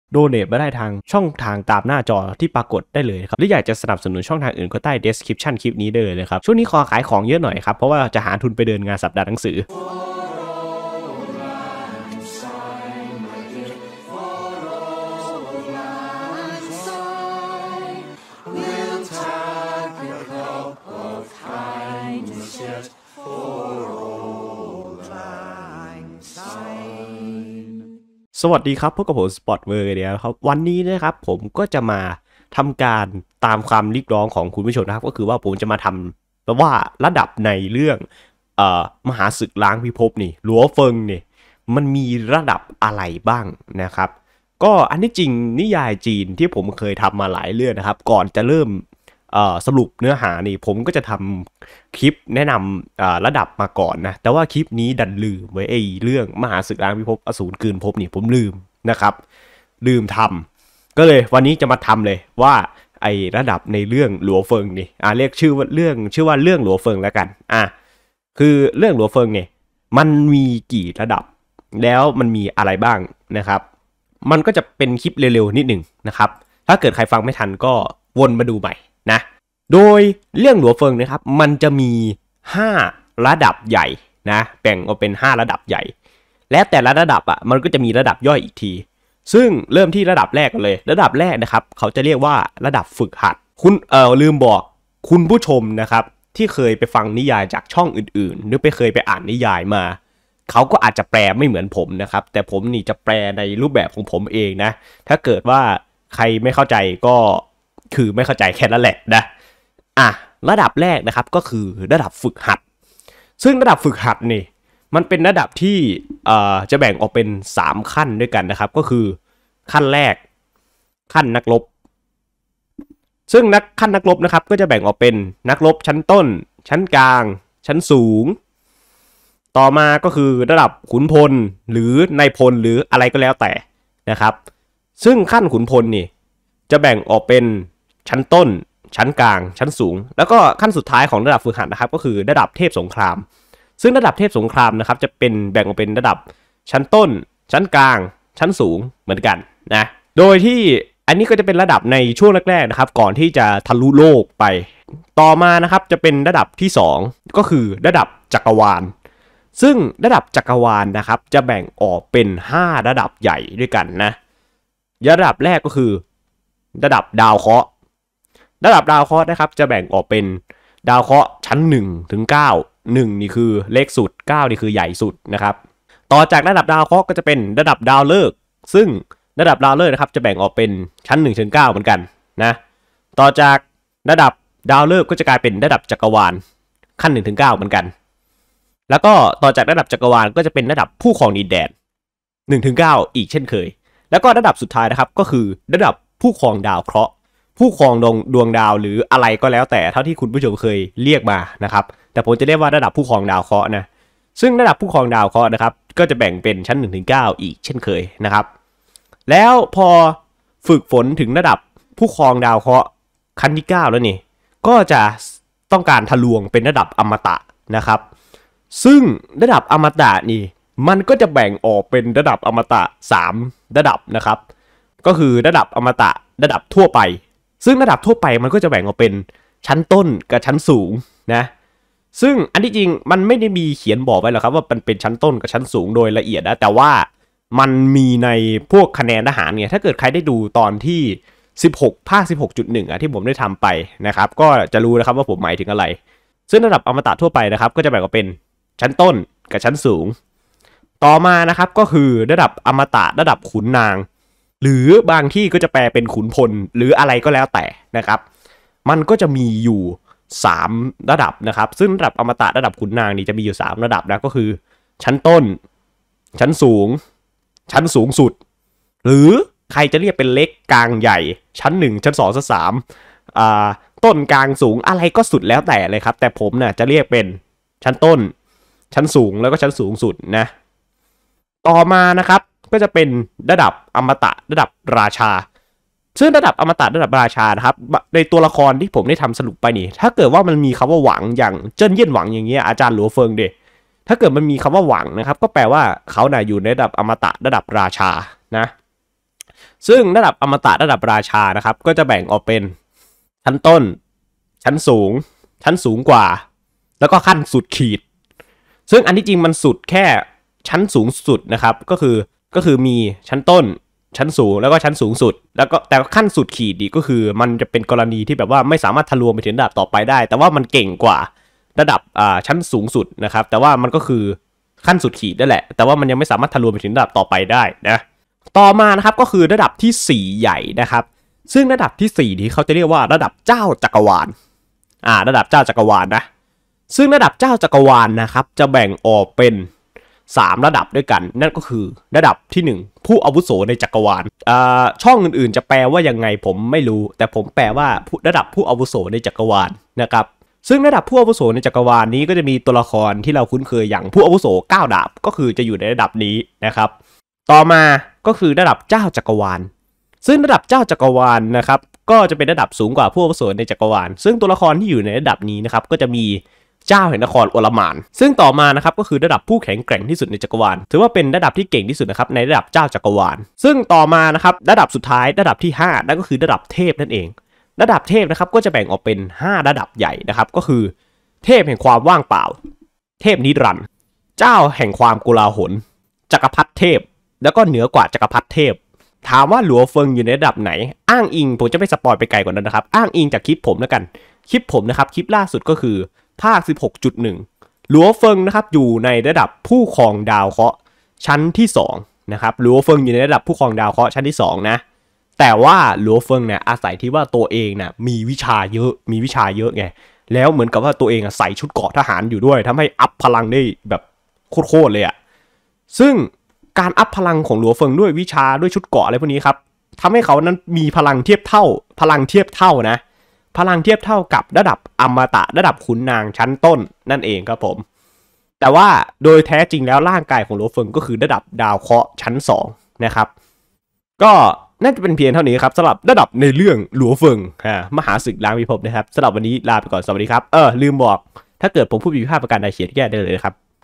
แนะนำระดับพลังต่างๆในเรื่อง Swallowed Star อสูรกลืนภพ(มหาศึกล้างพิภพ) | Spot World
คลิปแนะนำระดับพลังต่างๆในเรื่อง Swallowed Star อสูรกลืนภพ(มหาศึกล้างพิภพ) เป็นการแนะนำระดับพลังต่างในเรื่อง